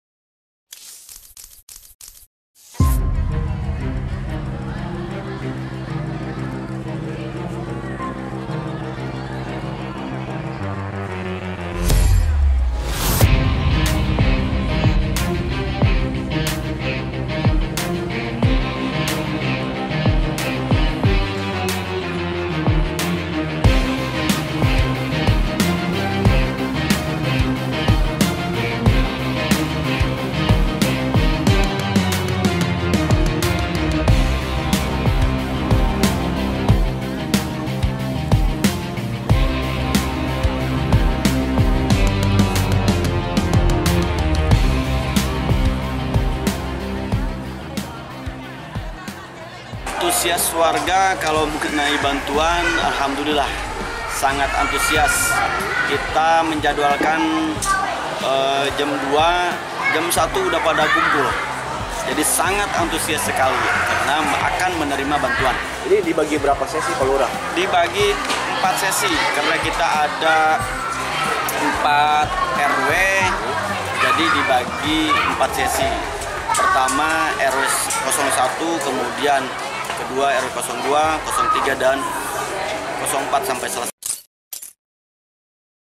Ratusan Warga Berdesakan dan Saling Serobot Antrean BLT BBM
Ratusan warga Kelurahan Kadu Agung, Tigakarsa, Banten rela berdesakan saat akan mencairkan dana bantuan sosial pascakenaikan harga BBM, Selasa (13/09/2022) siang. Bahkan warga yang ingin lebih dulu mencairkan BLT saling serobot antrean hingga terlibat saling dorong.

Kesal dengan aksi warga yang tidak tertib, petugas Desa Kudu Agung pun akhirnya meminta warga untuk kembali tertib agar pencairan bantuan sosial ini bisa berjalan lancar. Sebanyak 327 warga dari 4 RW di Kelurahan Kudu Agung antusias mencairkan BLT dan bantuan pangan non tunai sebesar Rp500 ribu.